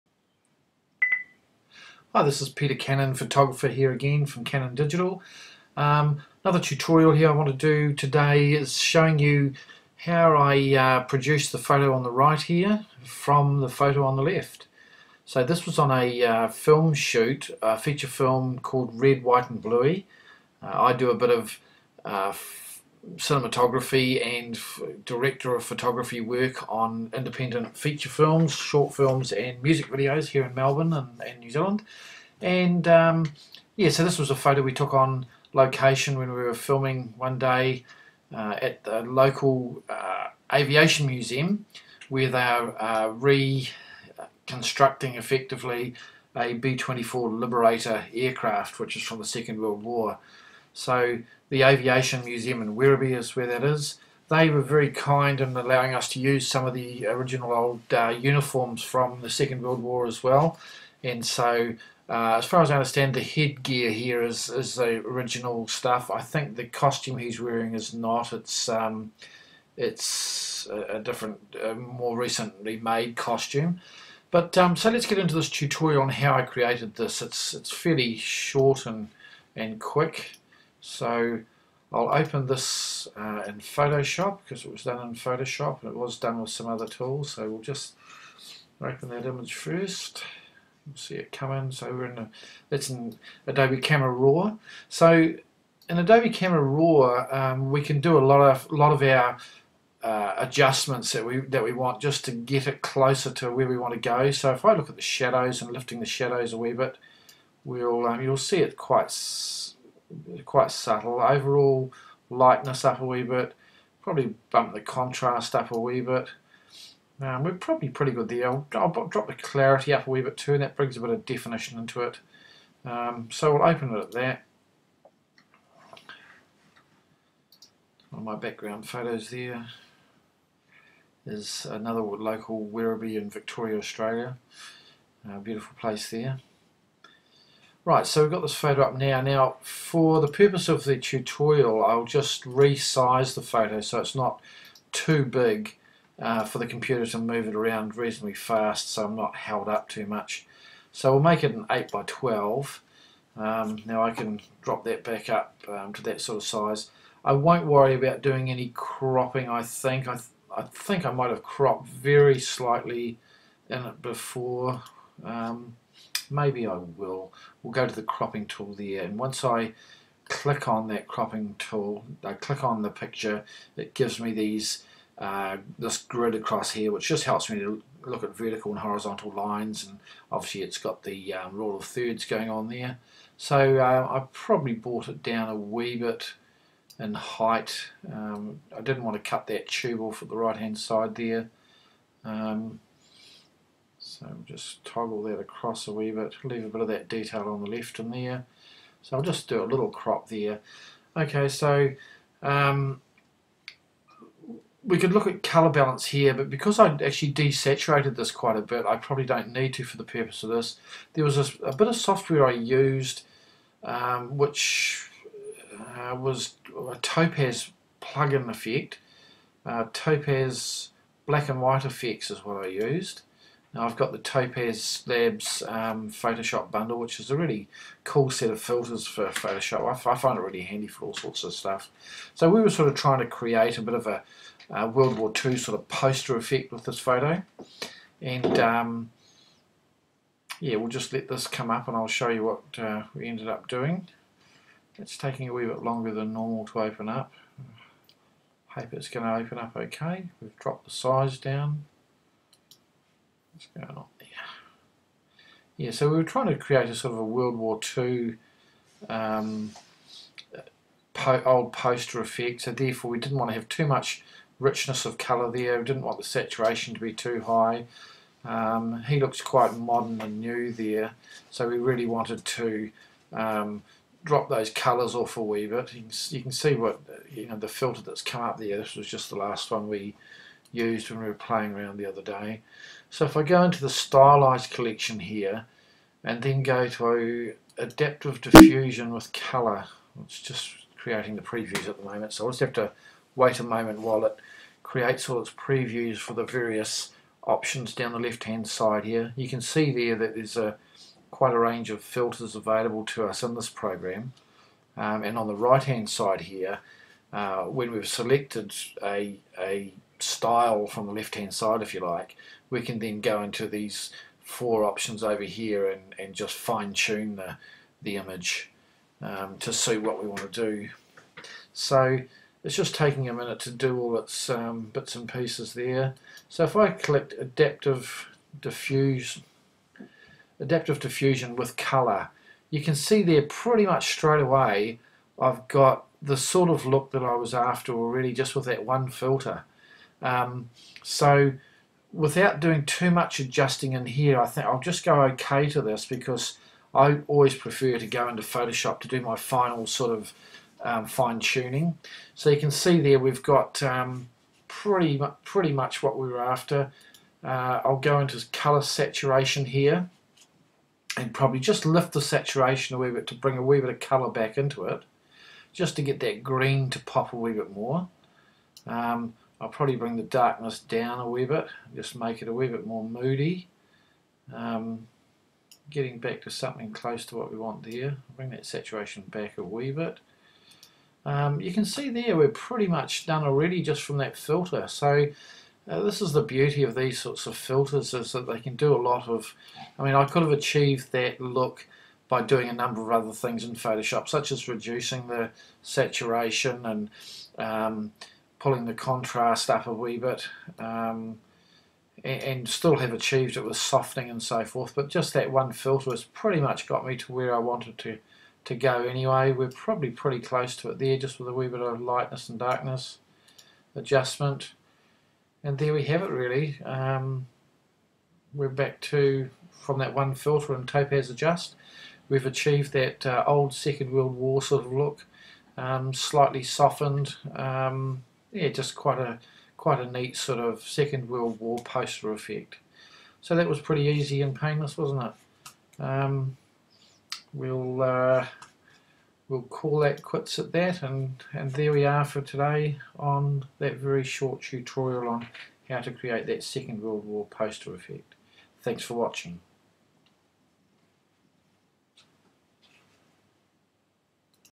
# 2 - World War 2 poster effect Tutorial in Photoshop & Topaz
Tutorial on creating a WW2 poster effect in Photoshop and Topaz.